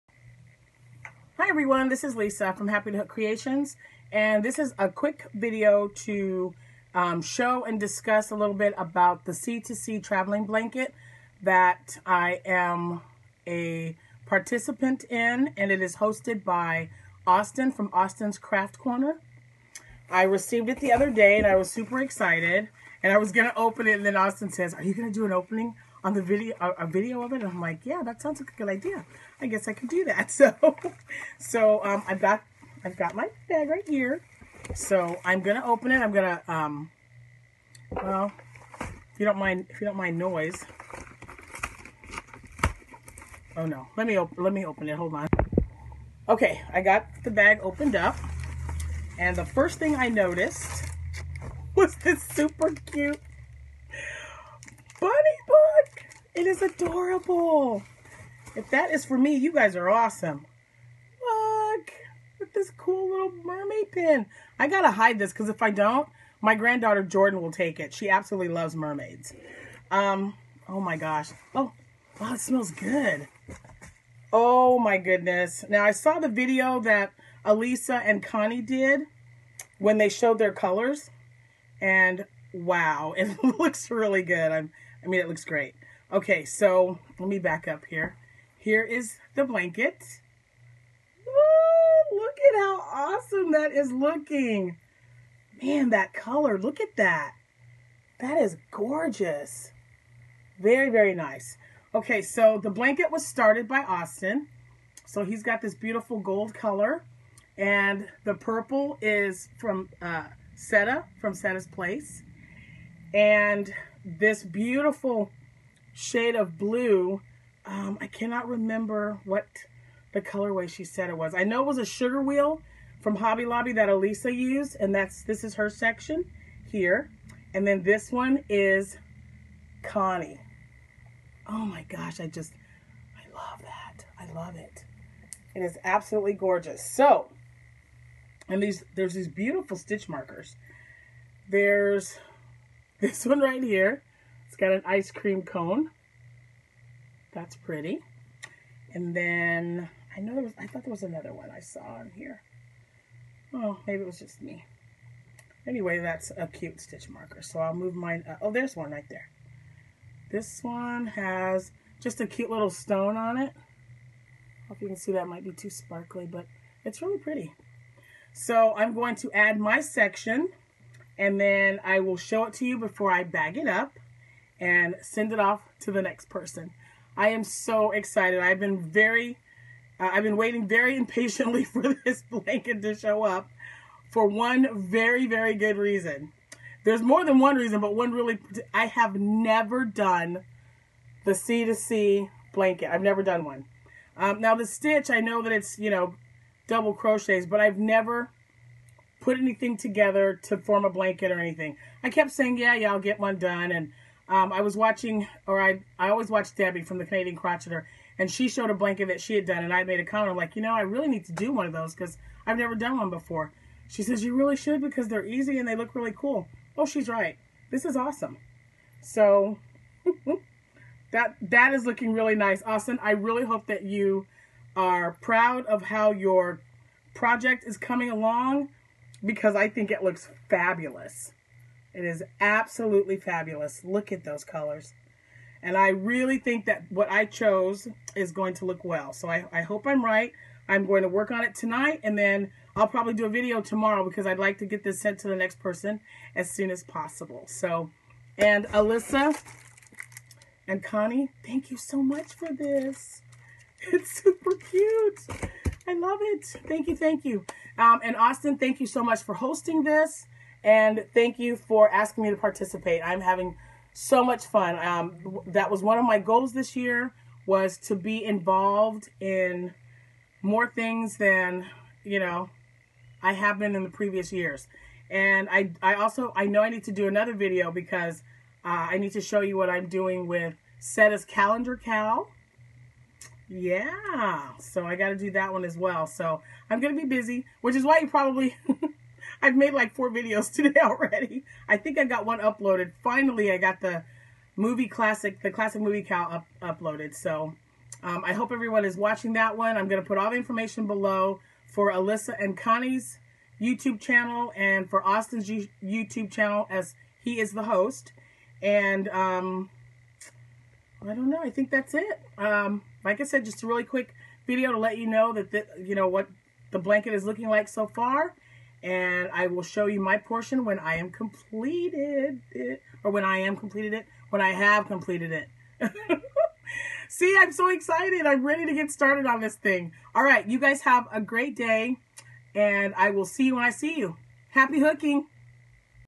Quick Chat/C2C Traveling Blanket
Showing off the progress of traveling C2C blanket hosted by Austin

Happy Mail
Lisa Larck
Happytohook Creations
Decatur,IL 62524

Happytohook Creations Chat